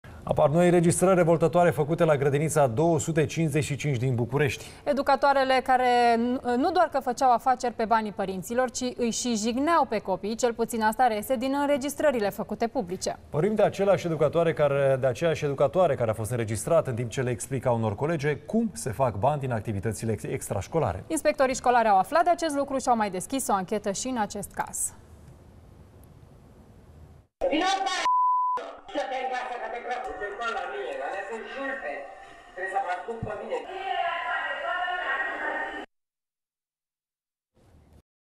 Noi înregistrări revoltătoare la o grădiniță din Capitală. Educatoarele fac afaceri pe banii
Apar noi înregistrări revoltătoare făcute la grădiniţa 255 din Bucureşti.

Educatoarele nu doar că fac afaceri pe banii părinţilor, ci îi şi jignesc pe copii. Cel puţin asta reiese din înregistrările făcute publice.

Vorbim de aceeaşi educatoare care a fost înregistrată în timp ce le explica unor colege cum se fac bani din activităţile extraşcolare.

Inspectorii şcolari au mai deschis o anchetă în acest caz.